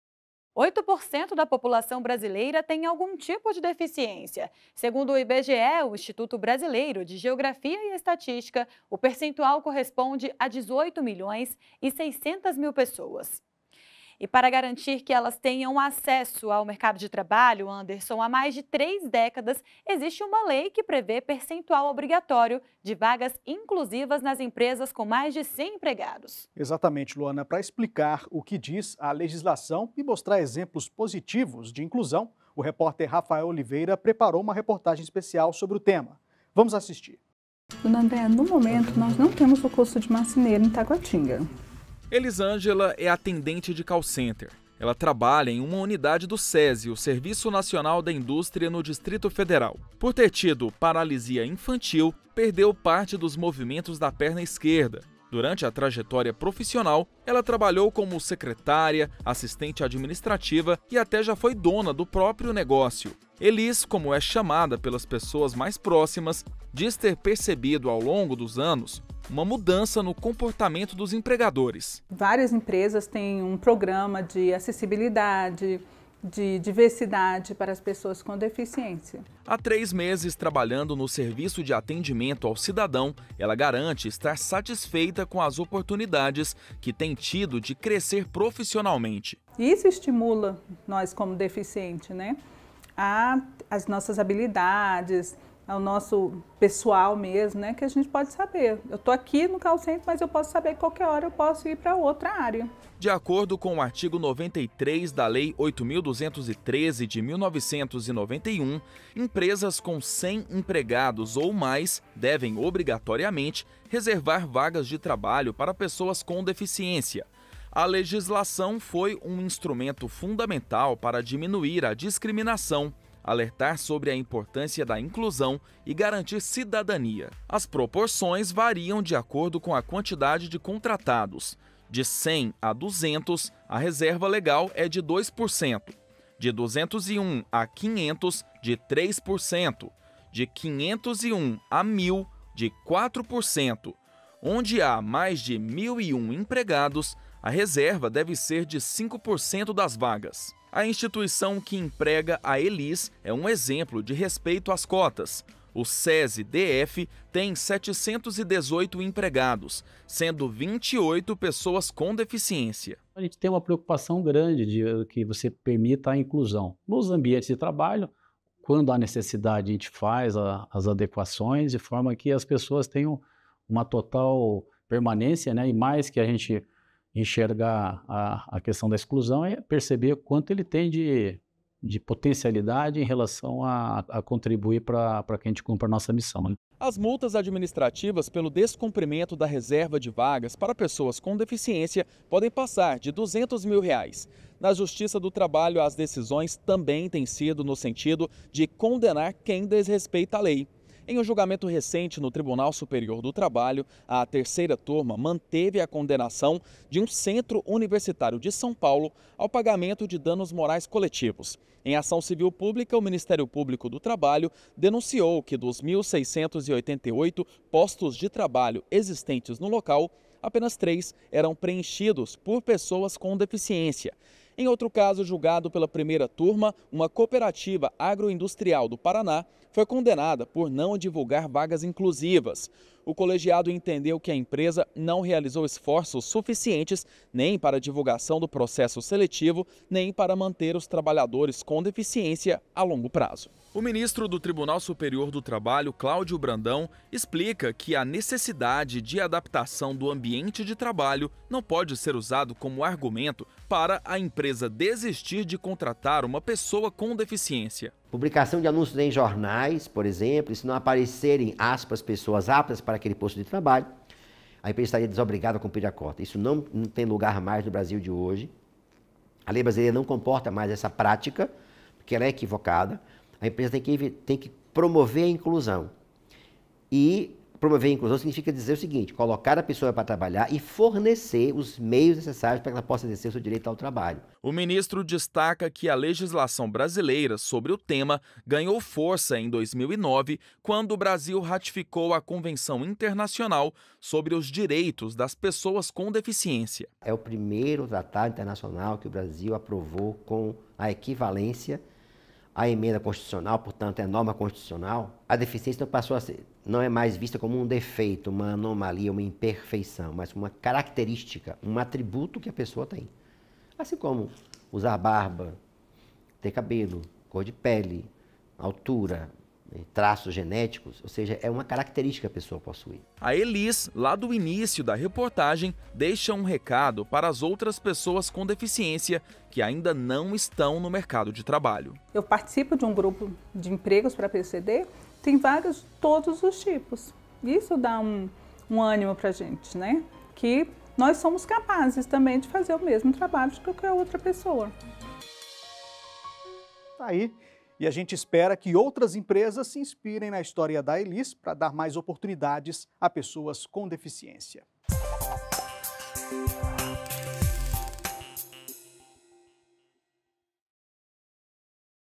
Leis de inclusão podem ser cumpridas com capacitação e remoção de barreiras
De acordo com o artigo 93 da Lei 8.213/1991, empresas com cem empregados ou mais devem, obrigatoriamente, reservar vagas de trabalho para pessoas com deficiência./ A legislação foi um instrumento fundamental para diminuir a discriminação, alertar sobre a importância da inclusão e garantir cidadania. Conheça bons exemplos de inclusão no mercado de trabalho.

O TST Tube é o canal oficial do TST no YouTube.